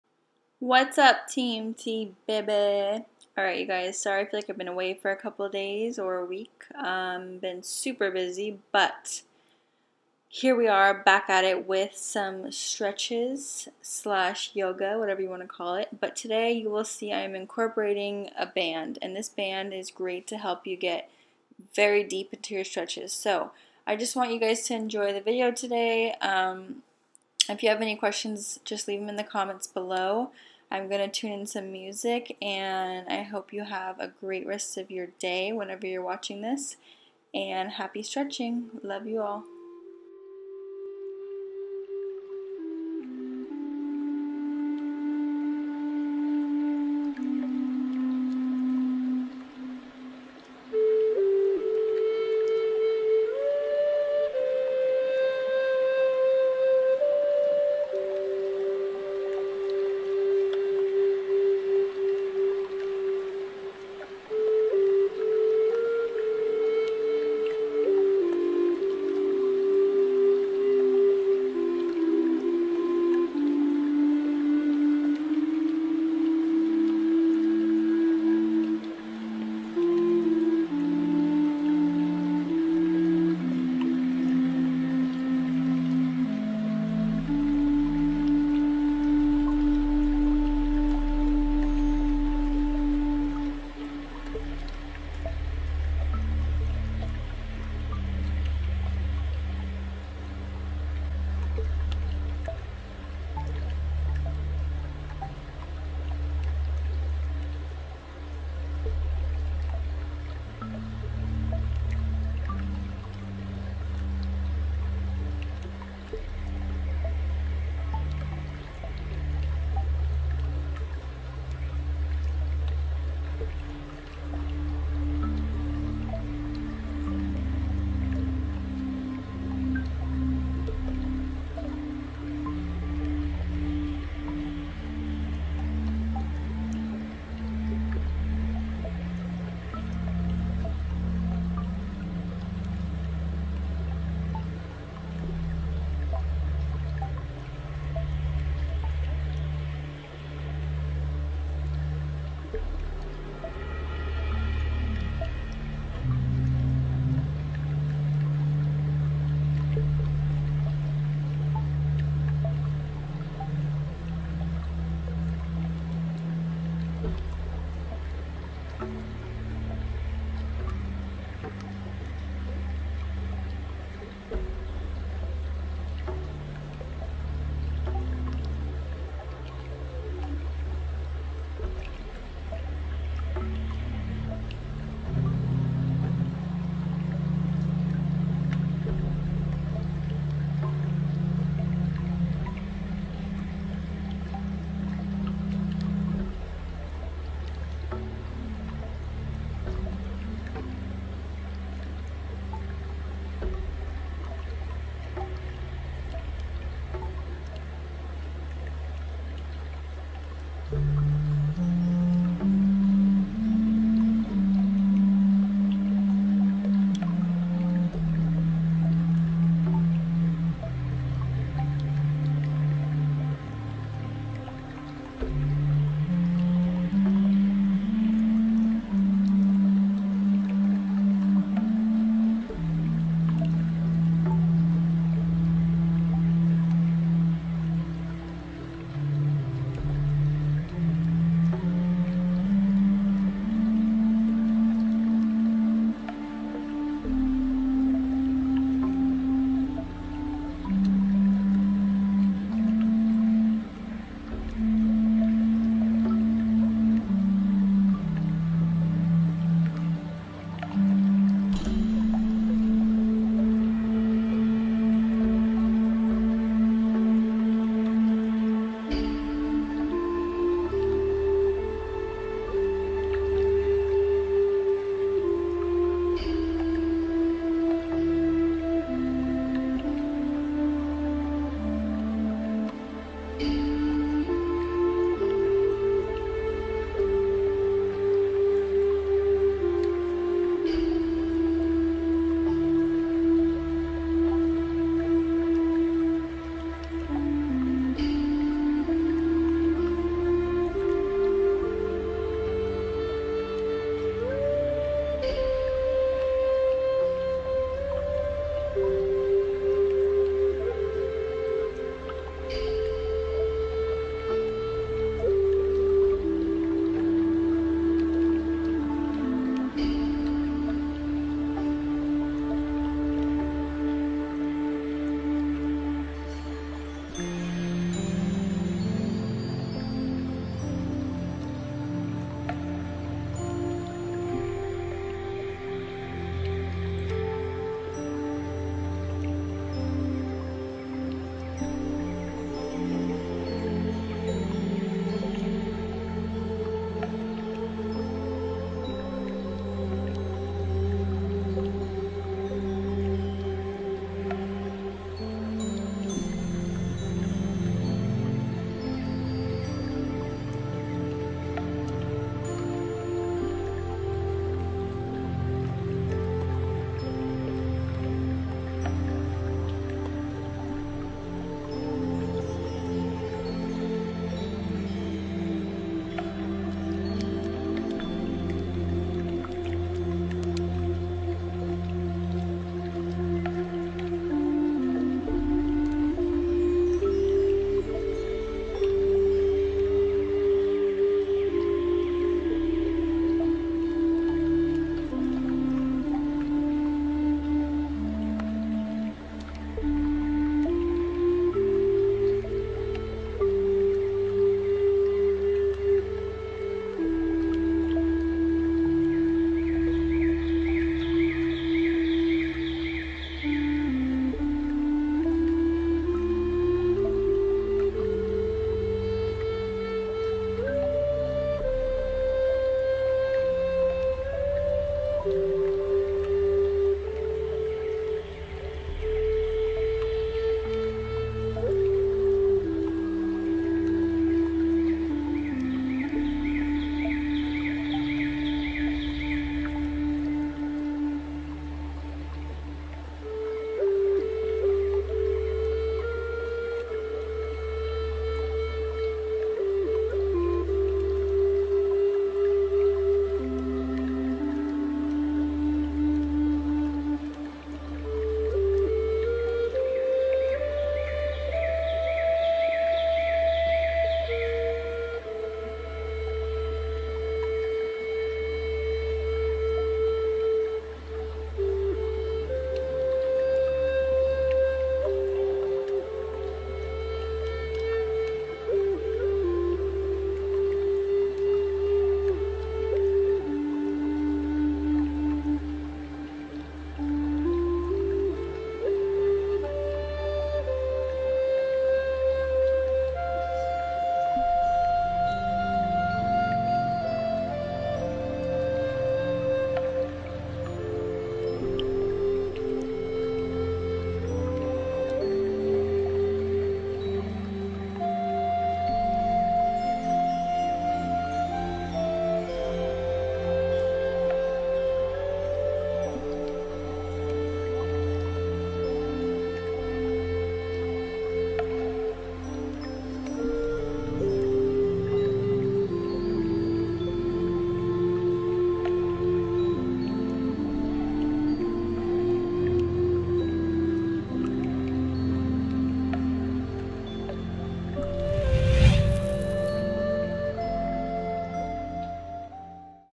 FULL BODY STRETCHES WITH BAND! DEEPER STRETCHES! 10 MIN!
HEY TEAM TBABY!!
STAY HEALTHY AND KEEP THOSE IMMUNE SYSTEMS HIGH!
LOVE YOU ALL!